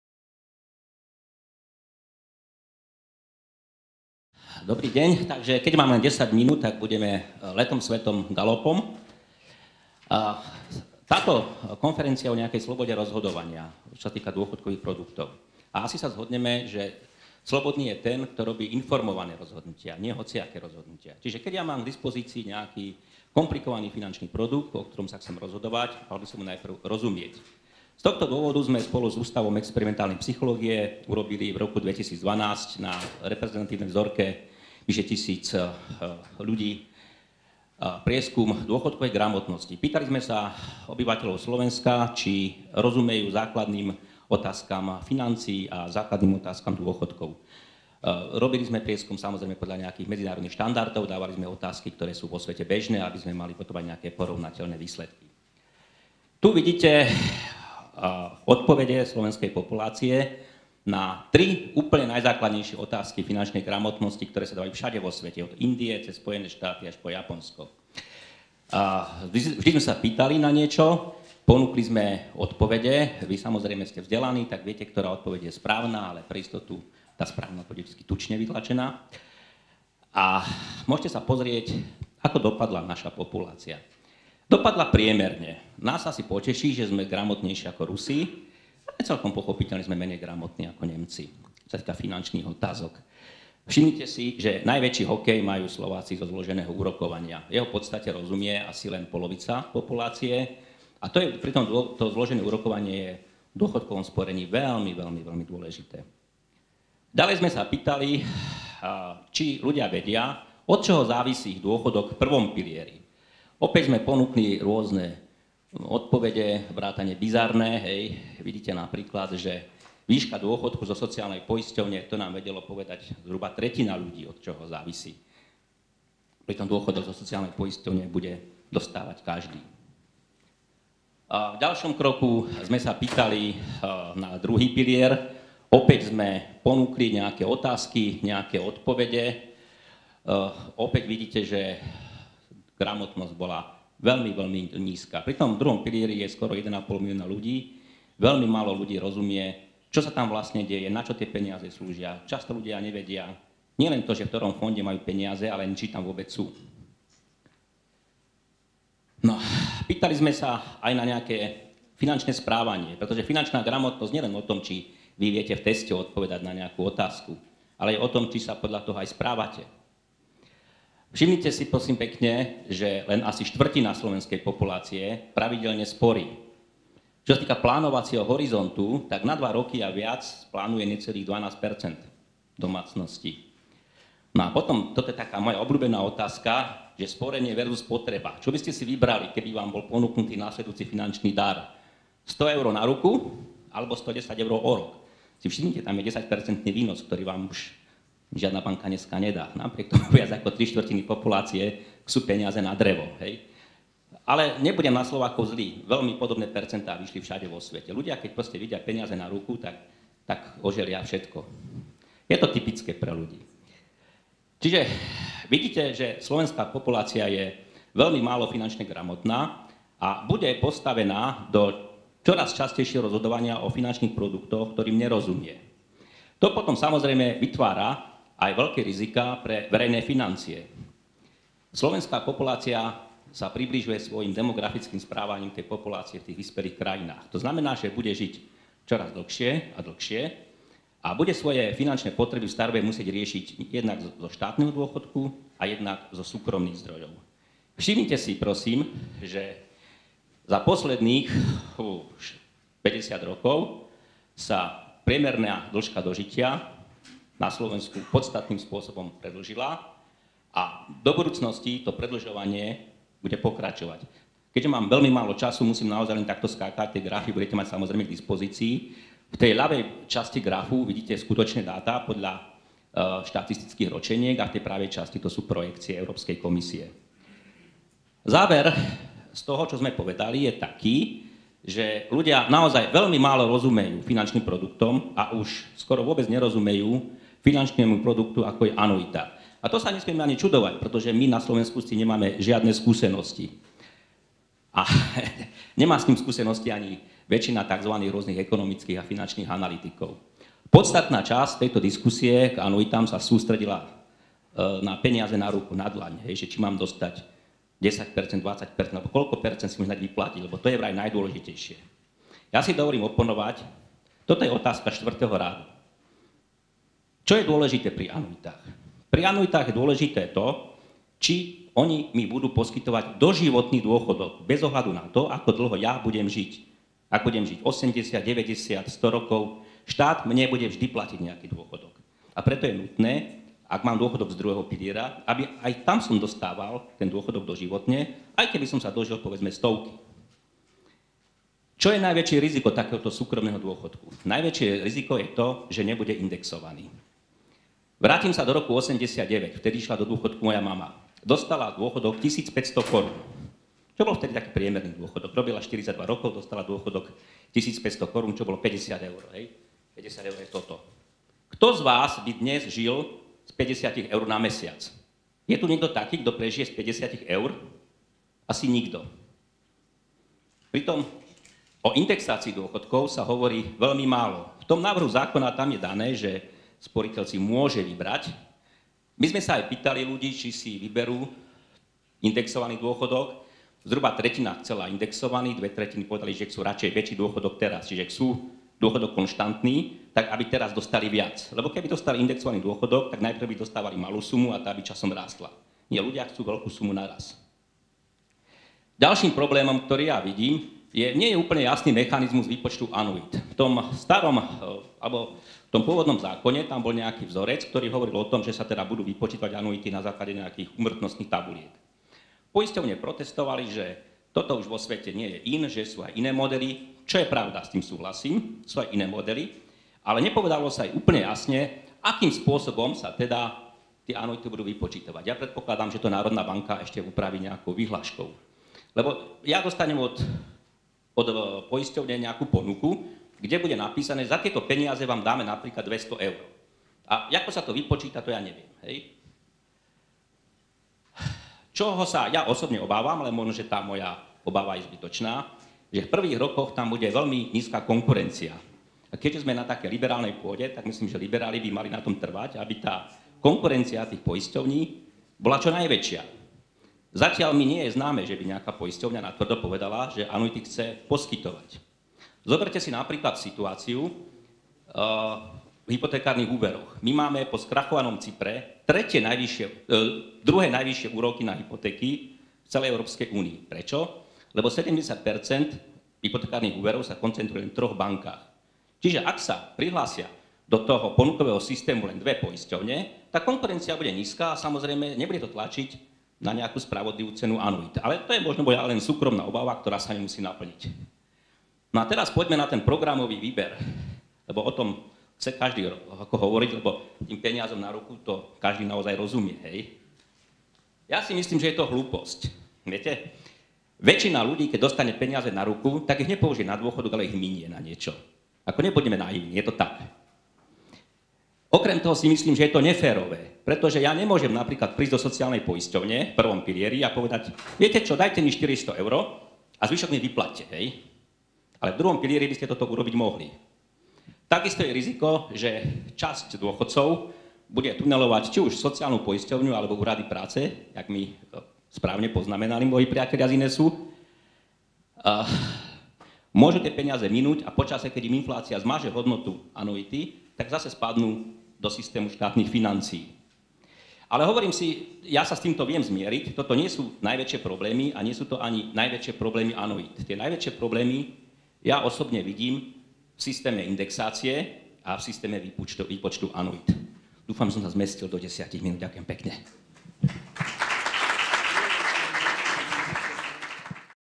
Vladimír Baláž - O obmedzení prístupu k úsporám v II. pilieri
Prednáška Vladimíra Baláža (SAV) na konferencii OBMEDZENIE PRÍSTUPU K ÚSPORÁM V II. PILIERI -- ÁNO alebo NIE?, ktorú organizoval INESS dňa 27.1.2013 v Bratislave.